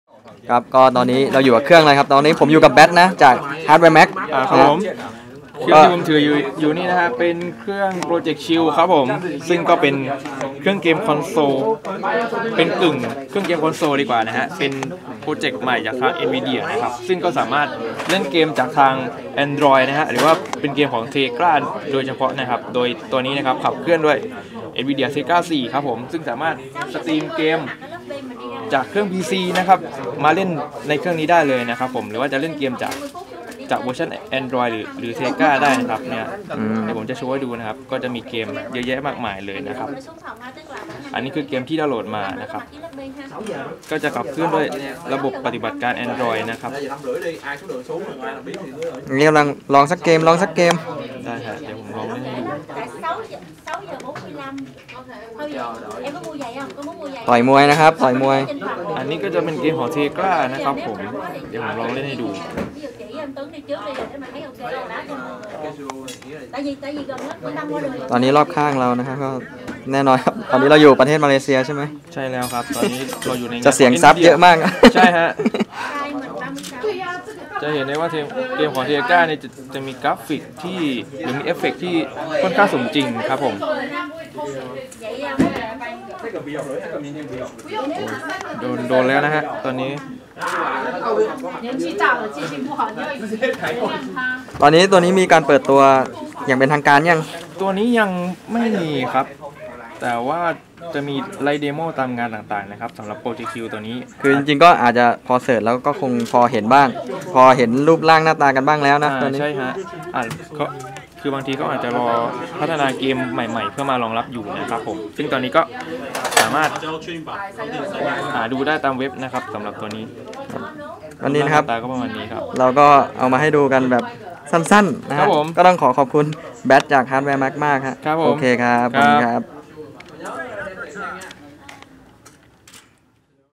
สัมผัสแรก NVIDIA Project Shield
คลิปนี้ก็เป็นคลิป แบบสั้นๆ เฉพาะกิต ใฟ้มองมองเห็นถึงรูปลักษณ์ และการเล่นเกม ของ NVIDIA Project Shield จริงๆทาง CompleteTV ก้อยากจะนำเสนอแบบเต็มๆเหมือนกัน แต่ว่าสื่อให้ความสนใจตัวนี้ค่อนข้างมาอก ทำให้เราสามารถที่จะลองเล่น ลองสัมผัสจริงๆ ตอนที่เลิกงานแล้วครับ ยังไงก็ลองชมกันดูครับ แล้วคอยติดตามกันว่าจะมีการเปิดตัวอย่างเป็นทางการเมื่อไหร่ และราคาเท่าไหร่กันแน่.